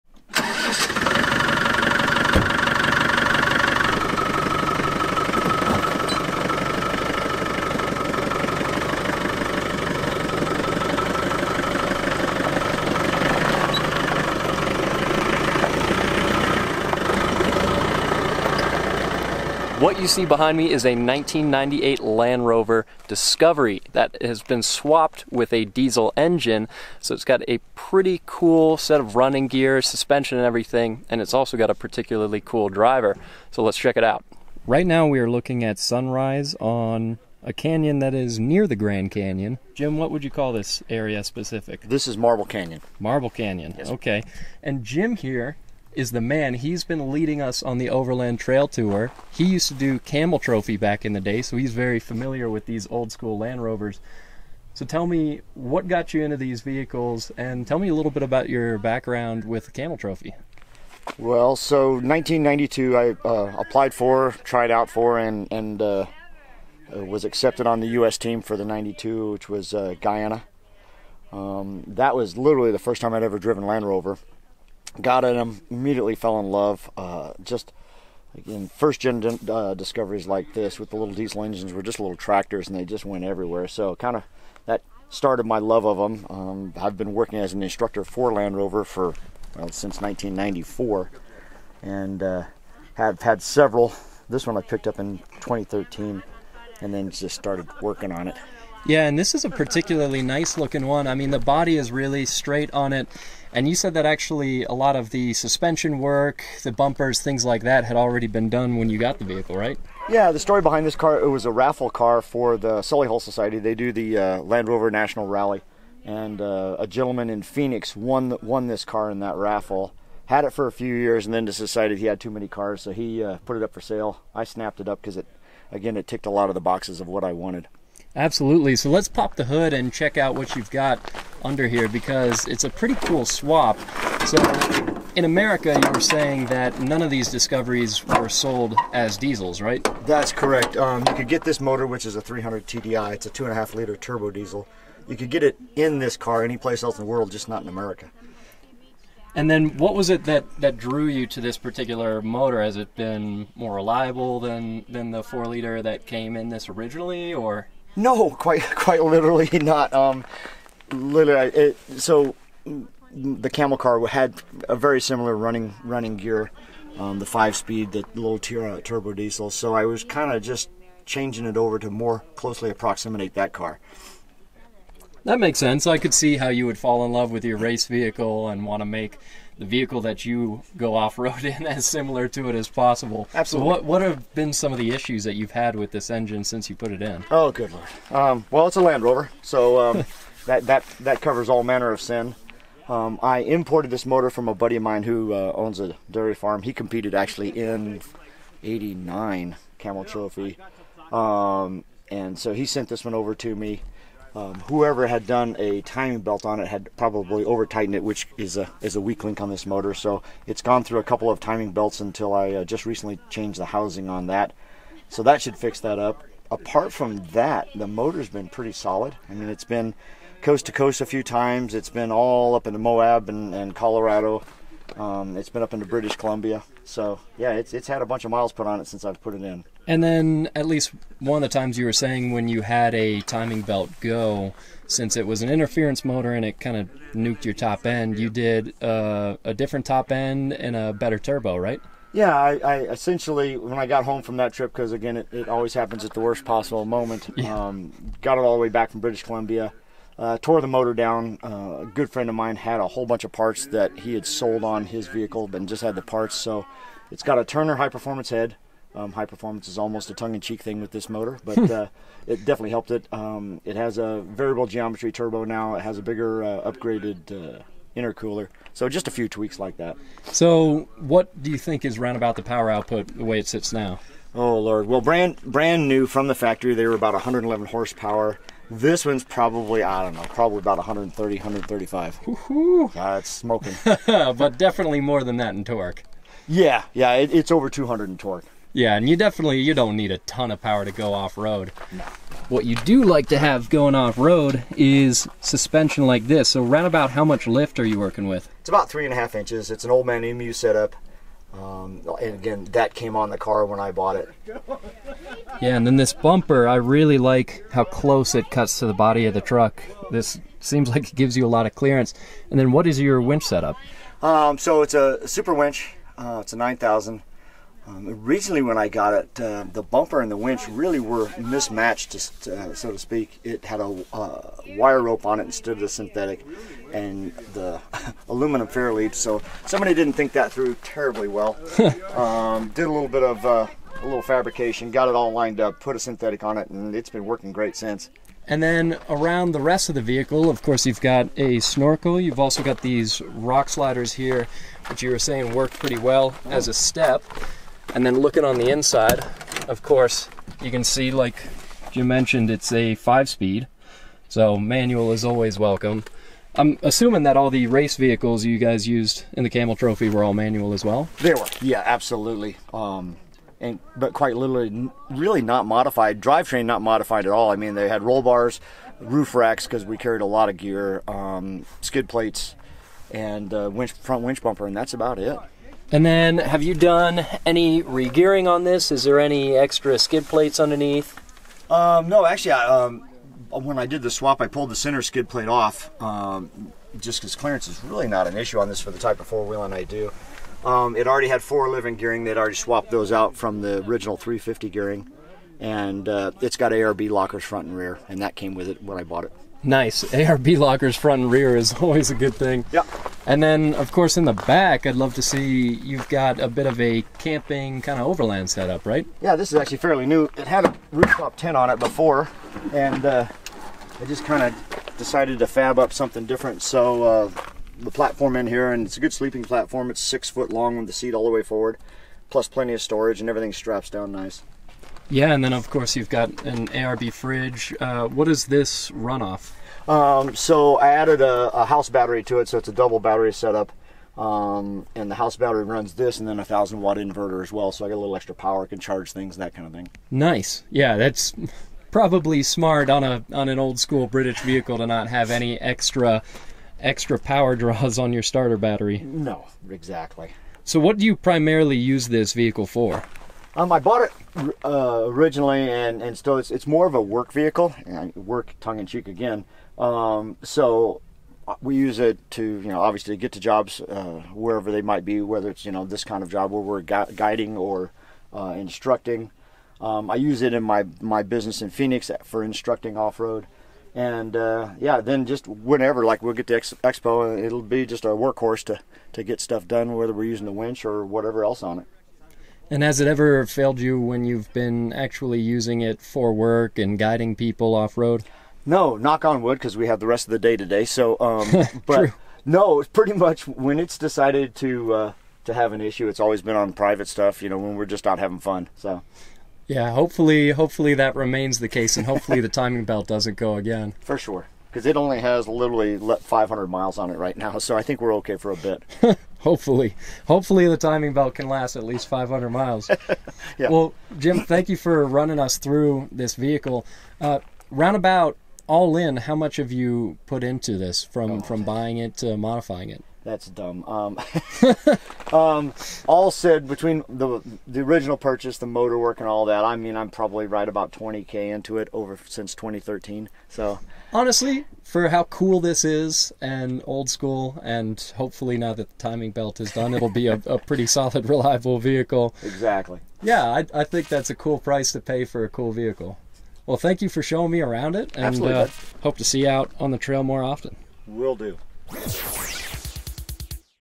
They Didn't Sell This AMAZING Land Rover Discovery In The U.S. So I Built My Own!
In this video we interview Jim, a veteran of the 1992 Land Rover Camel Trophy. This is his discover, and how he built it!